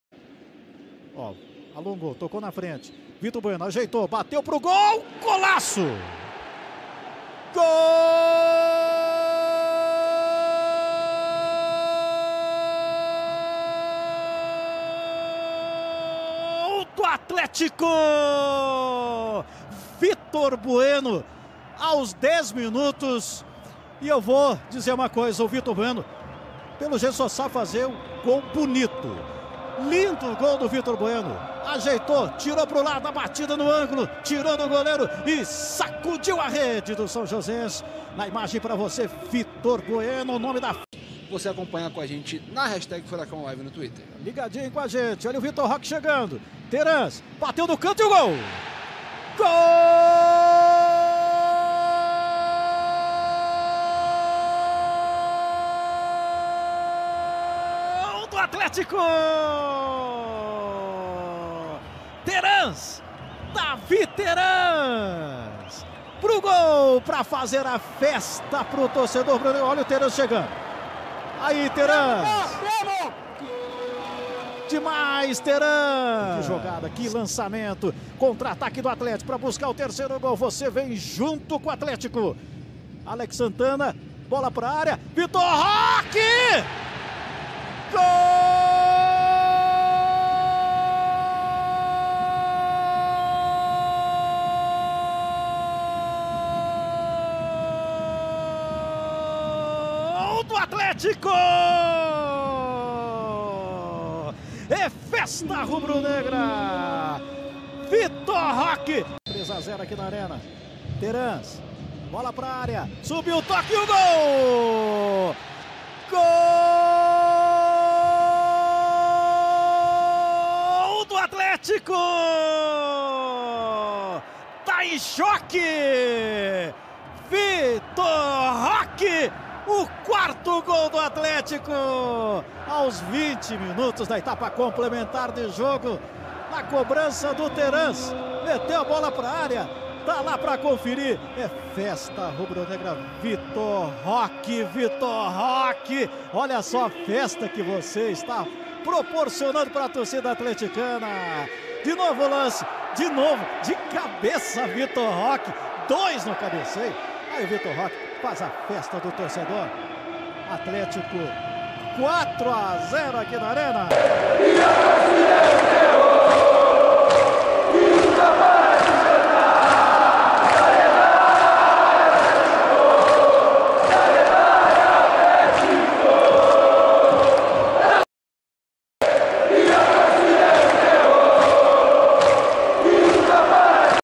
Athletico goleia o São Joseense e vai encarar o Maringá na semifinal
O Athletico passou com tranquilidade pelo São Joseense e está nas semifinais do Campeonato Paranaense. Neste domingo, o Furacão goleou por 4 a 0, na Arena da Baixada, e fechou o confronto com um elástico 9 a 2 na soma dos dois jogos. Vitor Bueno e Terans abriram a vitória no primeiro tempo, e Vitor Roque marcou duas vezes para fechar a conta na etapa final.
O Athletico pega agora o Maringá nas semifinais, com jogo de ida no Willie Davids e a volta na Arena da Baixada.